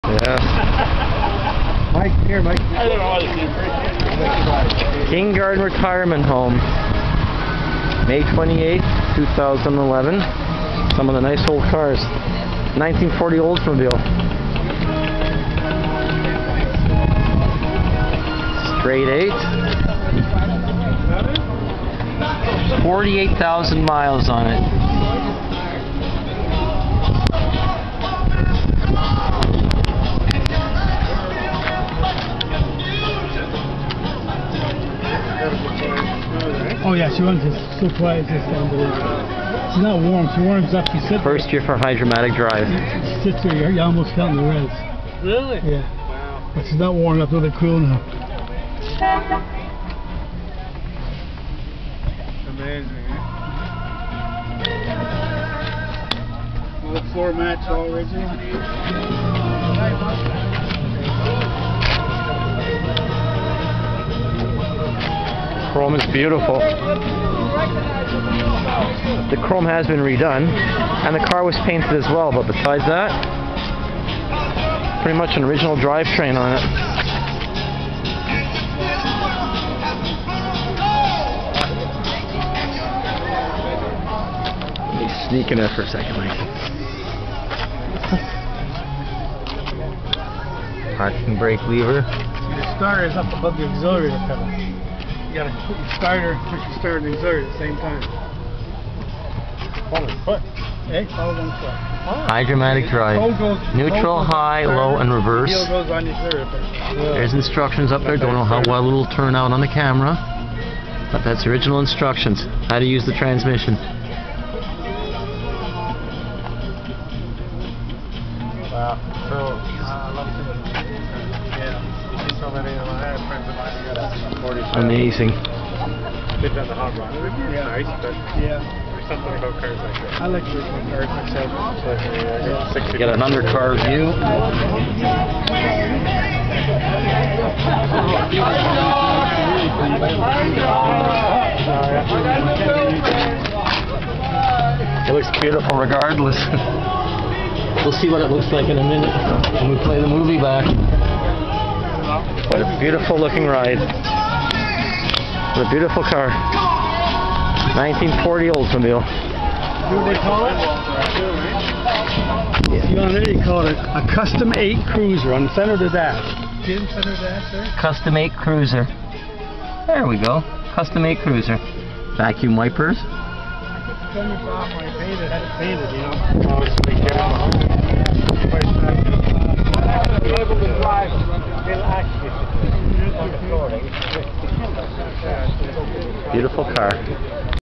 1940 Oldsmobile with Hydramatic
One of the interesting vehicles at the King's Garden Retirement Home Cruise seen was a 1940 Oldsmobile with the newly introduced Hydramatic automatic transmission.  This car is a 'survivor' car, the chrome has been redone in spots, and it does have new paint.  The interior is original and the owner has all the brochures that went with this time capsule.  The mileage is very low too. Underhood is mostly original too.  The spare is original as well.